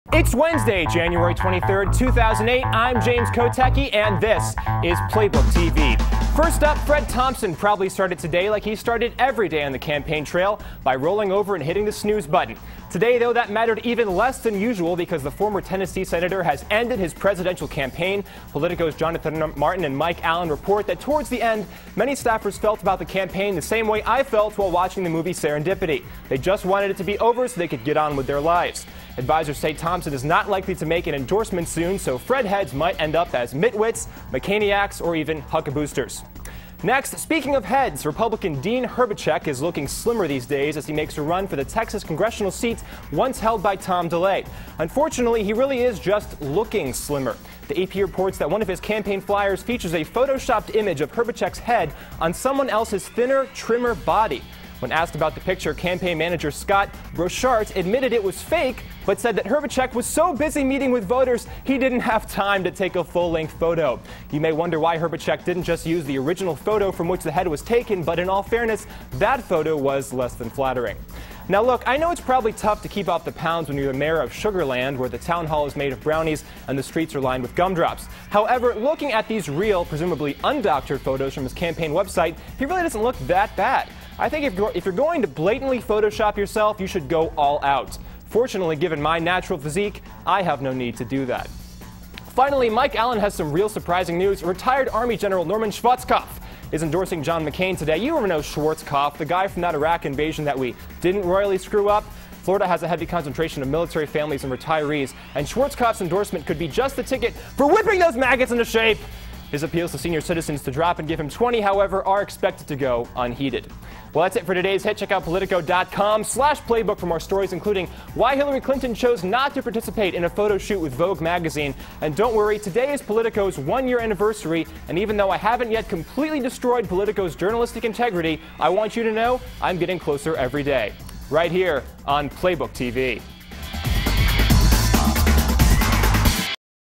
Congressional Candidate Fakes Photo of Himself
Fred Thompson drops out, a Congressional candidate's picture is Photoshopped, and Norman Schwarzkopf endorses John McCain.